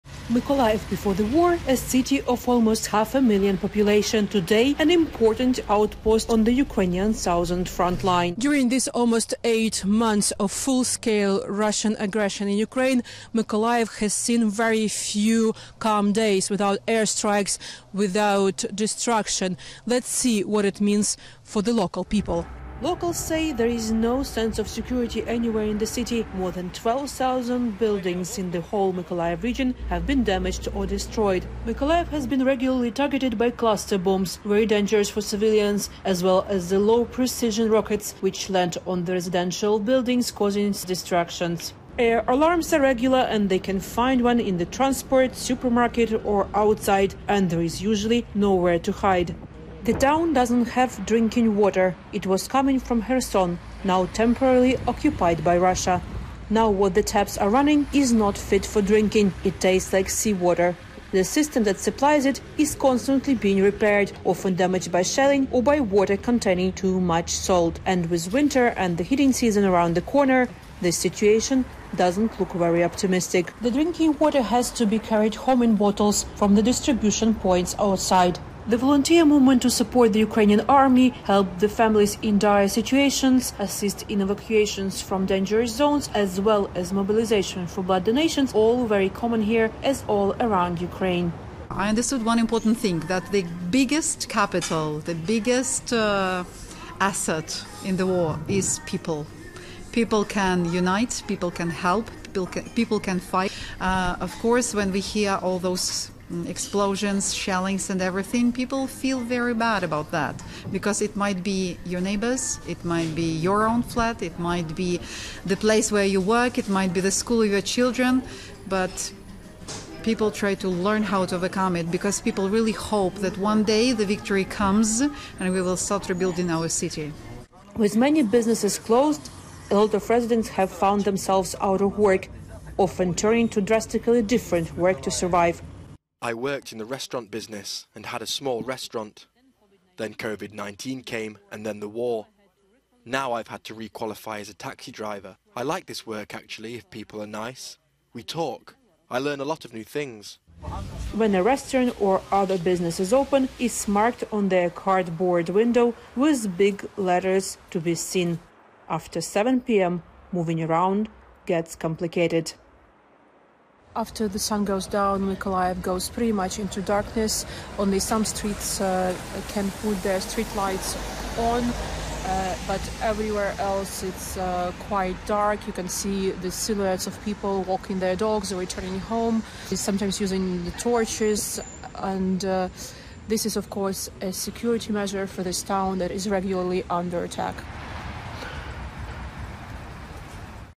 Ukraine war: Mykolaiv residents band together despite looming fears of attack
"The biggest capital, the biggest asset in the war is people can unite, people can help, people can fight," says one Mykolaiv resident.